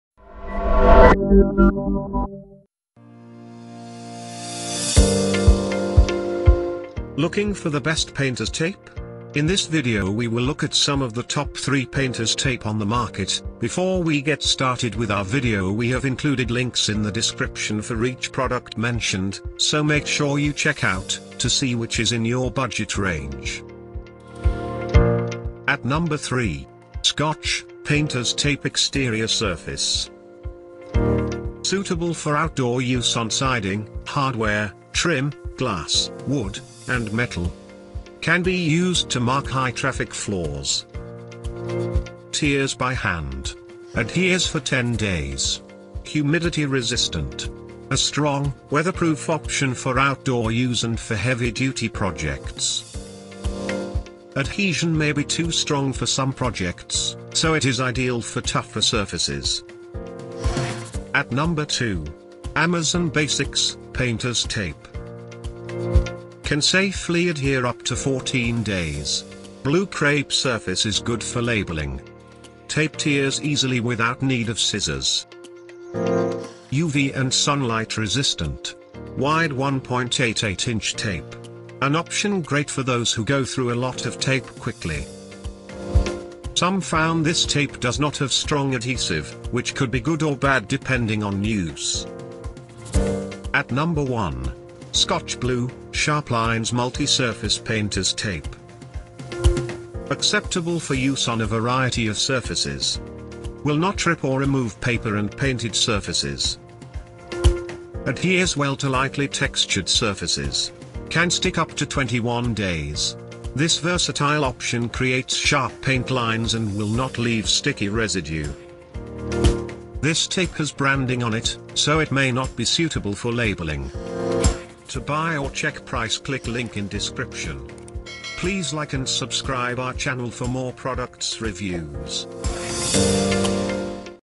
Best Painters Tape For Every Surface in 2023
🌐 ➡️ Best Painters Tape Reviews

1. ScotchBlue, Sharp Lines Multi-Surface Painter's Tape.
3vBg6NP

2. Amazon Basics, Painter's Tape.
ILHOyj

3. Scotch, Painter's Tape Exterior Surface.
JDM5M

0:00 Top 3 Best Painters Tape
0:25 Scotch, Painter's Tape Exterior Surface.
1:00 Amazon Basics, Painter's Tape.
1:33 ScotchBlue, Sharp Lines Multi-Surface Painter's Tape.